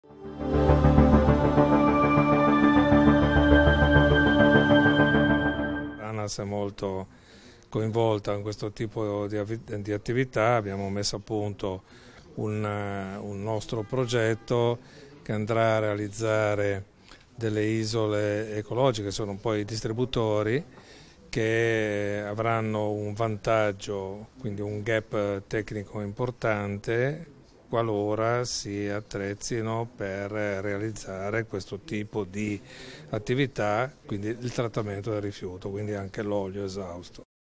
Anas, Gemme: “Messo a punto progetto per isole ecologiche”
Siglato protocollo di intesa Confindustria-Conou